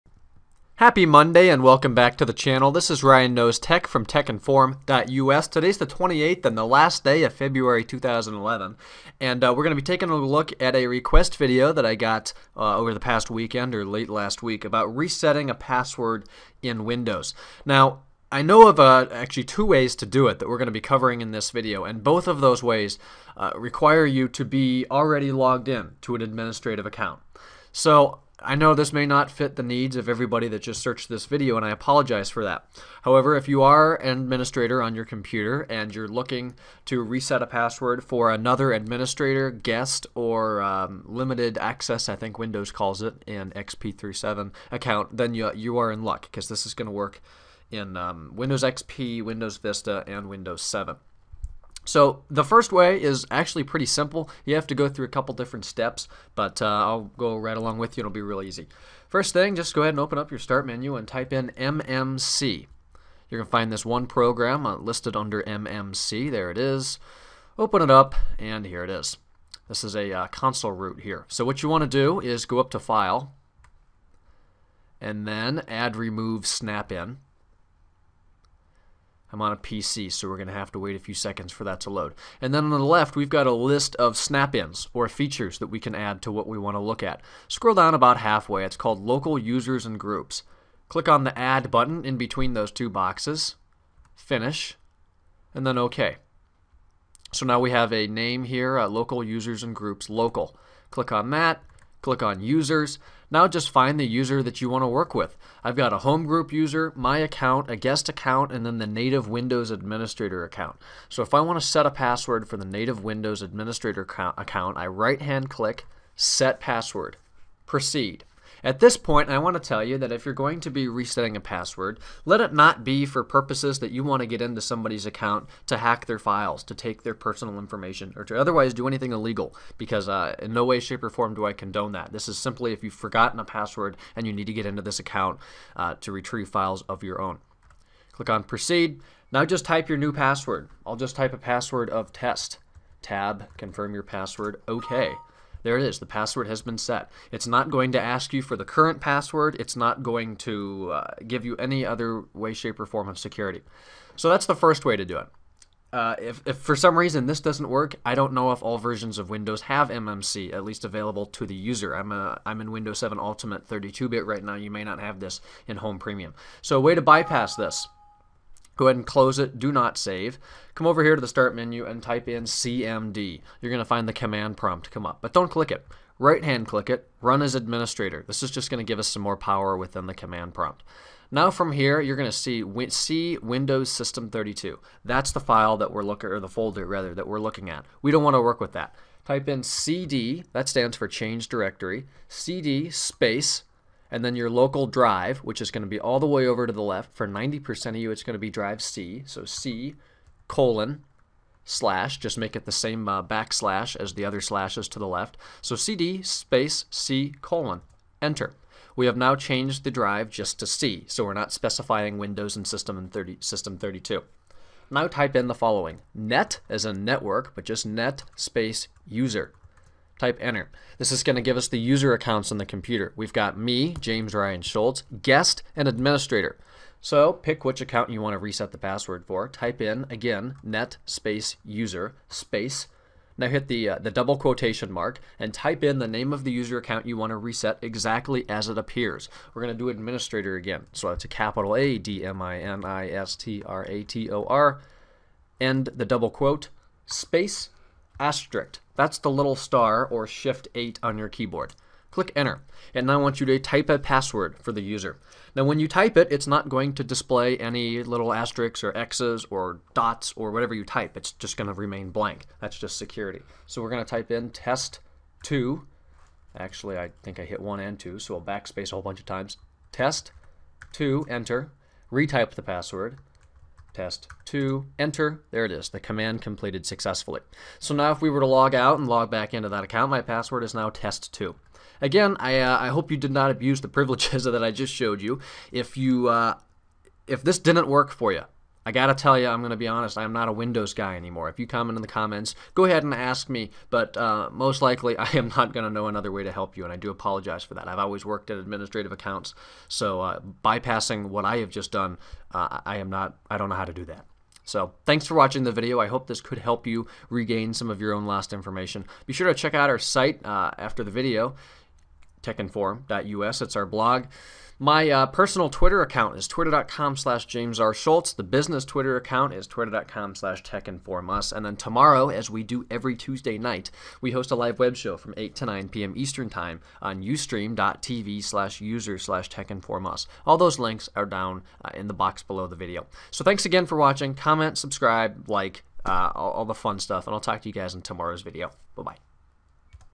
Windows Password Reset (From Admin Account)
This video shows TWO ways to do this, if the first way doesn't work, wait for the second!  The two tricks should work on any Windows account, if you are logged in yourself as an Administrator.  They should work on Windows XP - Windows 7.